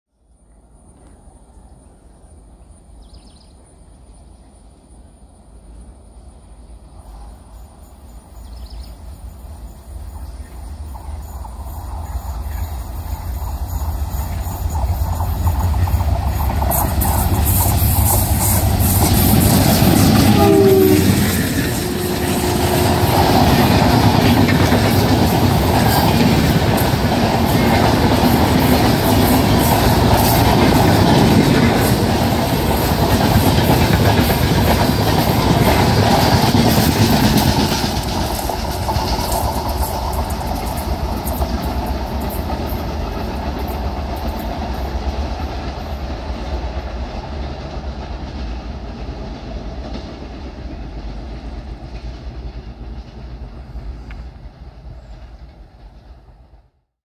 WDM3D Powered Duronto Express Skip Udupi In Style
Mumbai  - Ernakulam Duronto Special zooms past Udupi powered by WDM3D/11365 + WDM3D/11383 from Kalyan Shed.

Captured on 19 September 2021.